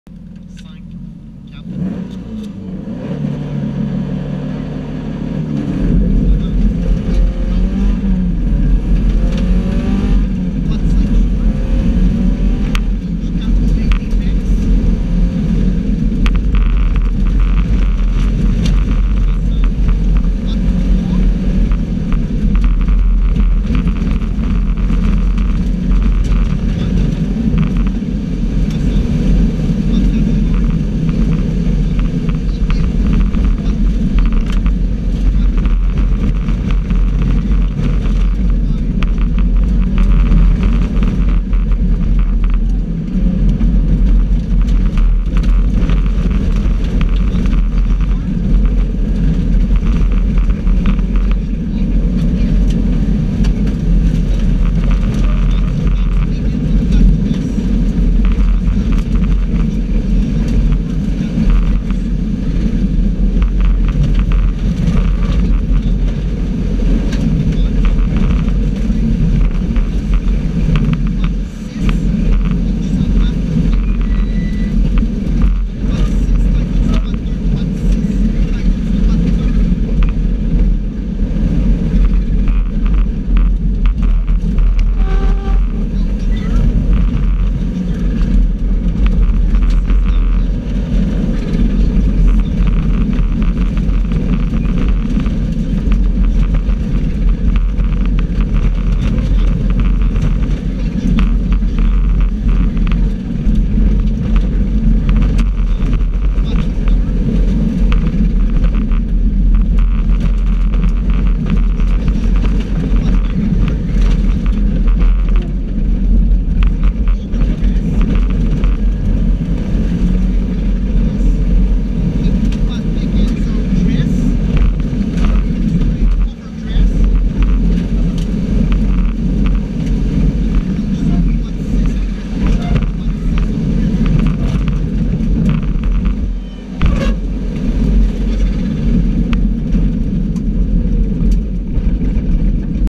Sylvain Vincent Onboard Baie des Chaleurs Saut du rang 5
Enjoy the drive along side Sylvain Vincent on one of the fastest stage of the Rally Baie des Chaleurs.